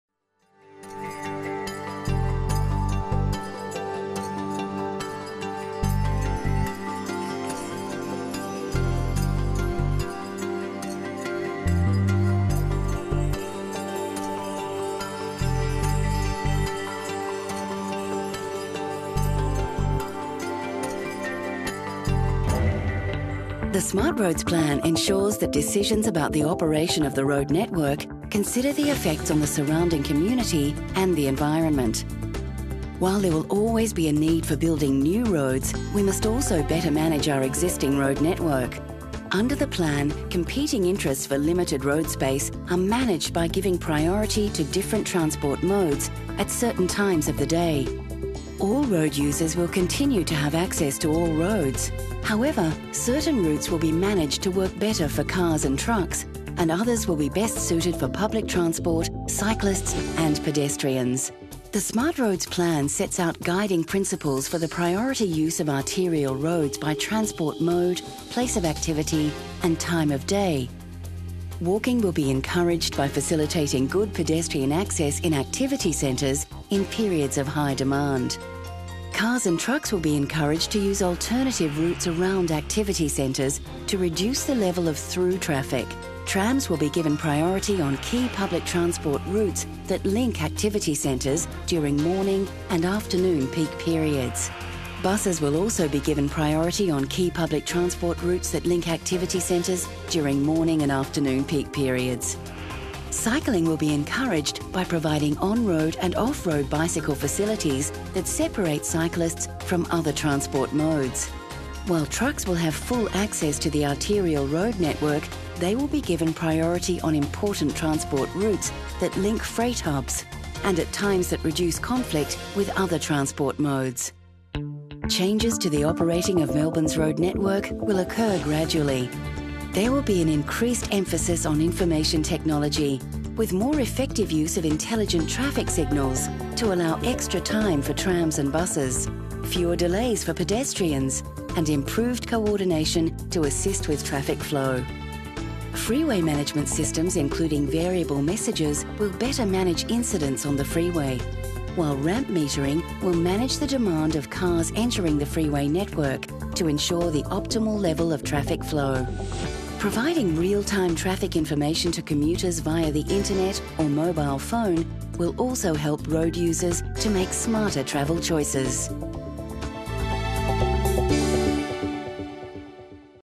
SmartRoads: Connecting communities
SmartRoads is an approach that is changing the way we use and operate our road network.